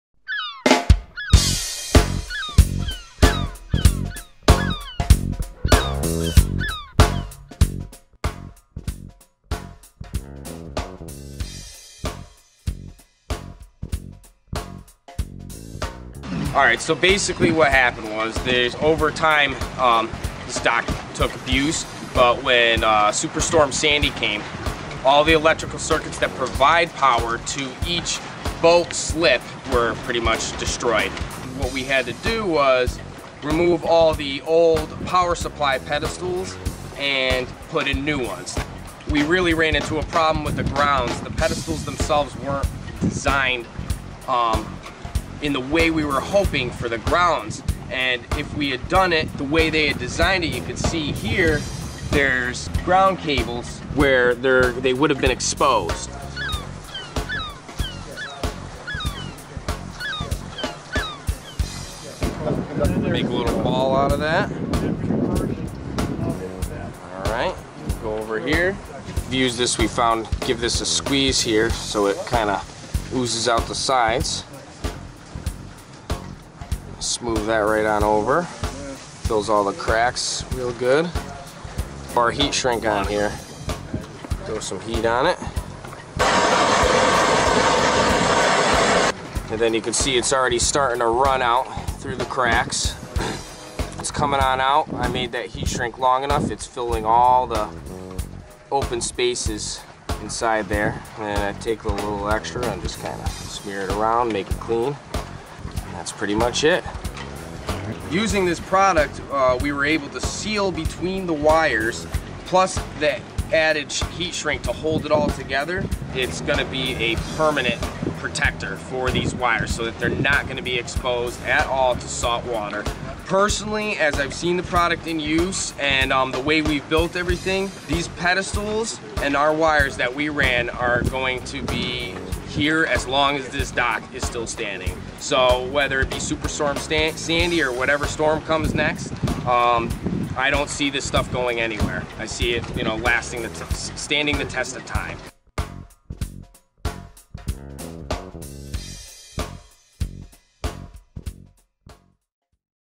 New London CT Marina Case Study
Super Storm Sandy damaged and destroyed marina docks and their power pedestals in 2012.  Watch how Stopaq 2100 Aquastop lends a hand in the rebuild to eliminate water ingress and seal leaks on low pressure leaks.